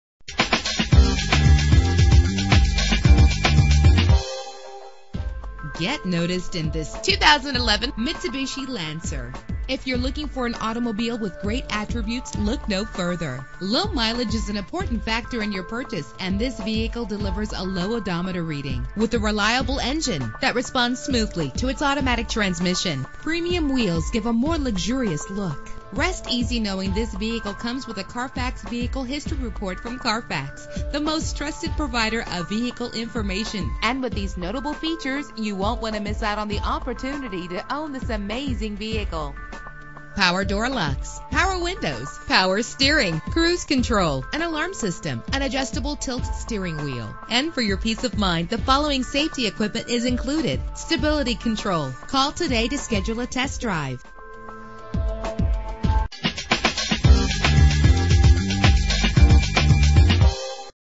2011 Mitsubishi Lancer - Beaverdale PA
Year: 2011
Make: Mitsubishi
Model: Lancer
Trim: GTS Sedan 4D
Engine: 4 cylinder 2.4 Liter
Transmission:
Color: Black
Mileage: 3675
Address:
1120 Beaver Run Avenue